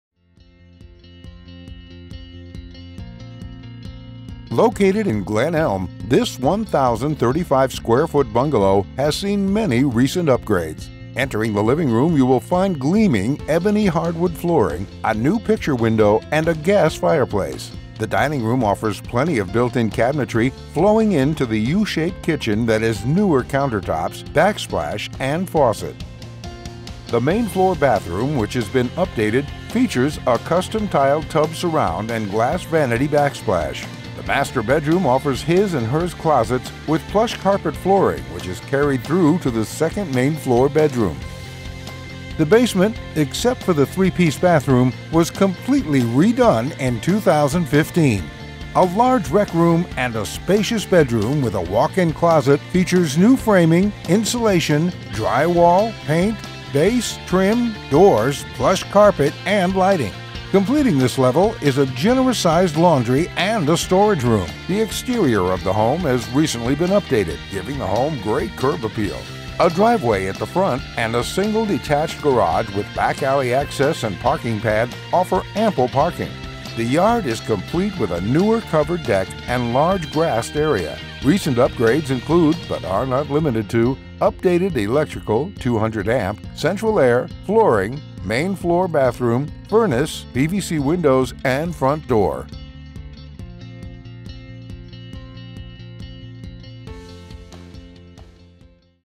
75 Rothwell Street, Regina, Saskatchewan
Located in Glen Elm this 1035 sq.ft. bungalow has seen many recent upgrades. Entering the living room you will find gleaming ebony hardwood flooring, a new picture window and gas fireplace. The dining room offers plenty of built in cabinetry flowing into the u-shaped kitchen featuring newer countertops, backsplash and faucet. The main floor bathroom has been updated featuring a custom tiled tub surround and vanity backsplash. The master bedroom offers his & hers closets and p...lush carpet flooring which is carried through to the second main floor bedroom. The basement (except for the 3 piece bathroom) was completely redone in 2015! A large rec room and spacious bedroom (with walk in closet) feature new framing, insulation, drywall, paint, base, trim, doors, plush carpet and lighting. Completing this level is a generous sized laundry and storage room. The exterior of the home has also been updated giving the home great curb appeal. A driveway at the front and a single detached garage with back alley access and parking pad offer ample parking. The yard is complete with a newer (covered) deck and large grassed area. Other upgrades include but are not limited to, 3/4” hardwood, newer paint, updated electrical (200AMP), central air, main floor bathroom, furnace (H/E), PVC windows and front door.